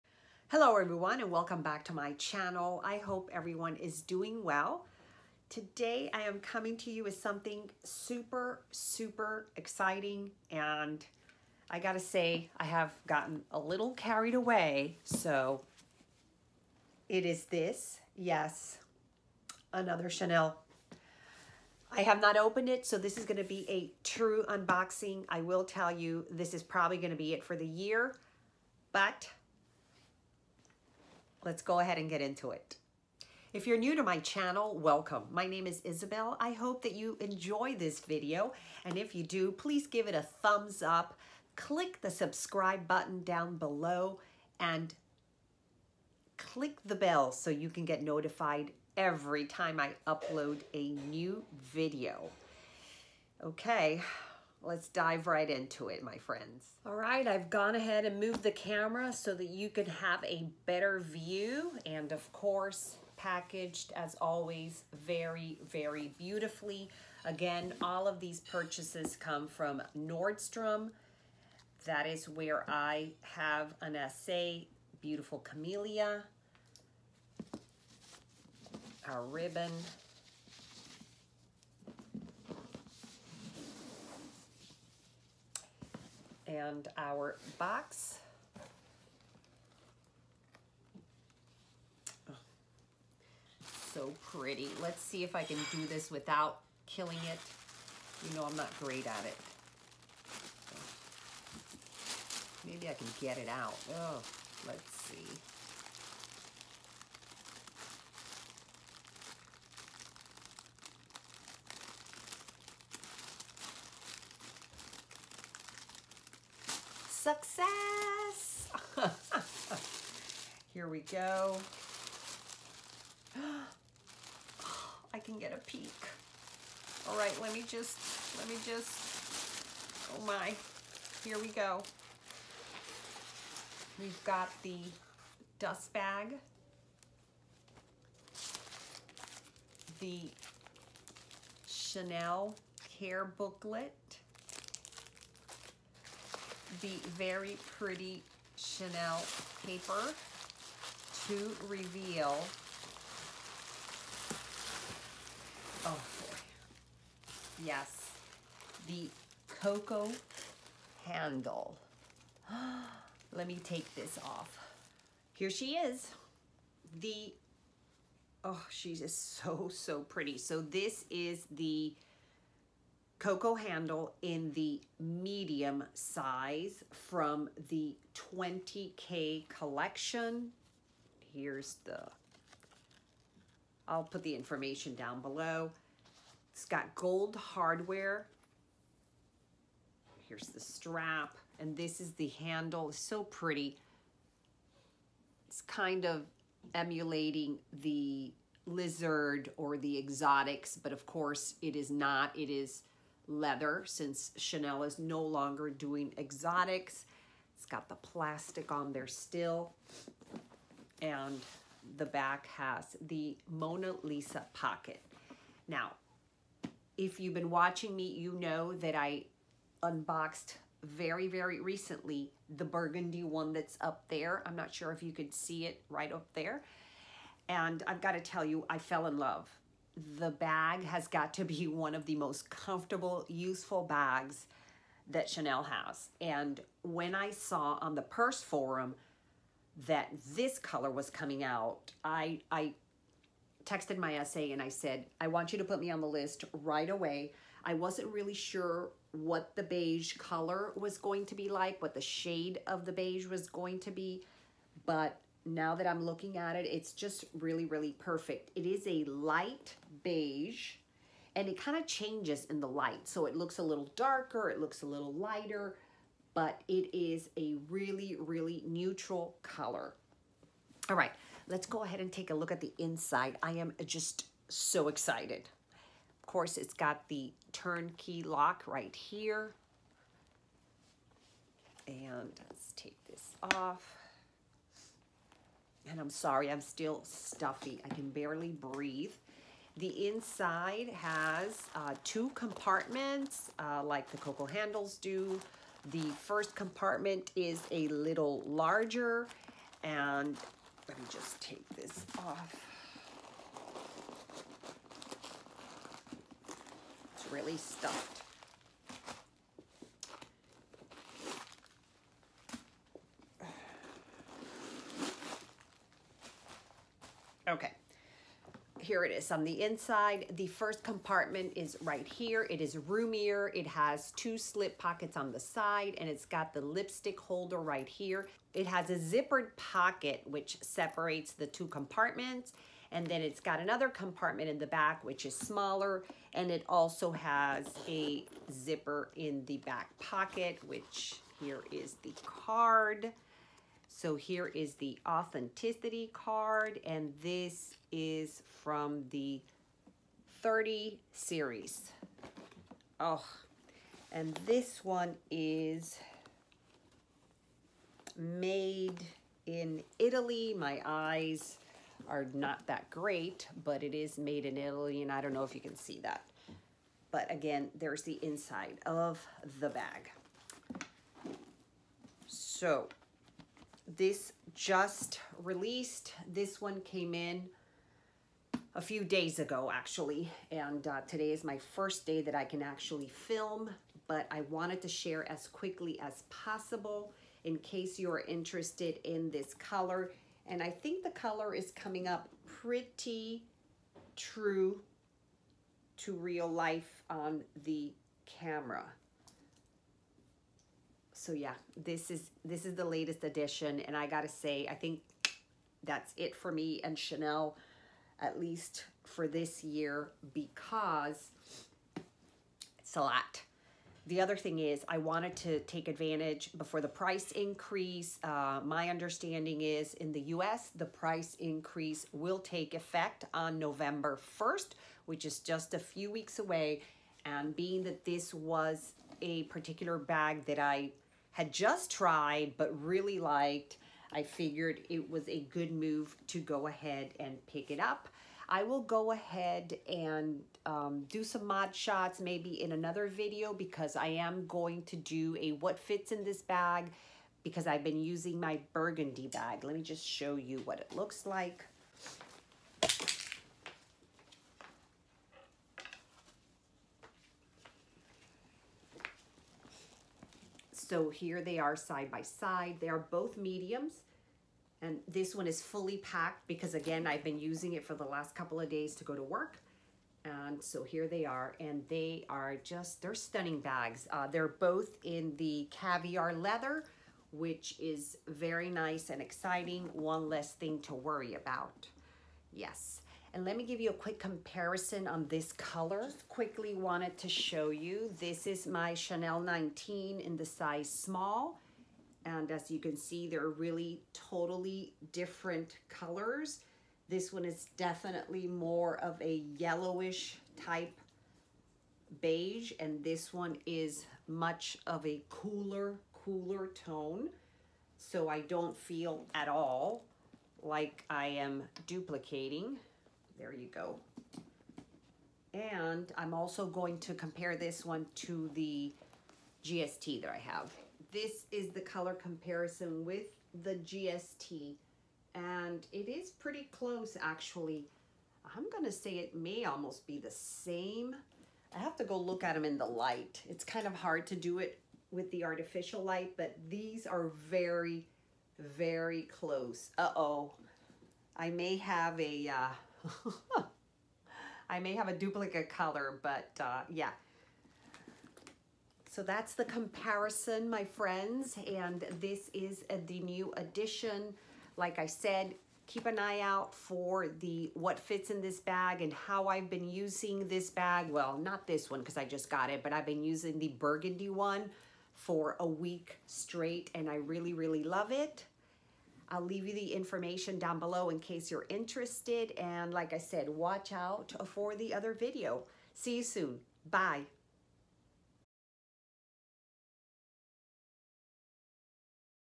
ANOTHER CHANEL COCO HANDLE UNBOXING 😱 | CHANEL 2OK COLLECTION
Let me know what you think - did I pull the trigger on this Chanel 20k beauty too soon???

Today's Make Up Look:
rimer: Victoria Beckham Priming Moisturizer
Foundation: Tom Ford Shade & Illuminate - Cool Beige 4.7
Setting powder: La Mer Loose Powder
Under Eye Powder: Pat McGrath
Bronzer: Tom Ford Glow Bronzer- 02 Terra
Blush: Wayne Goss -Blush Peony Duo - Shimmer Pink
Highlighter: Wayne Goss - Blush Peony Duo - Sweetened
Finishing Powder: N/ A
Lips: Pat McGrath  Soft Core & Divine Rose Gloss
Eyeshadow - Tom Ford - Shade & Illuminate Face & Eye Palette - Rose Cashmere
Mascara: Chanel Le Volume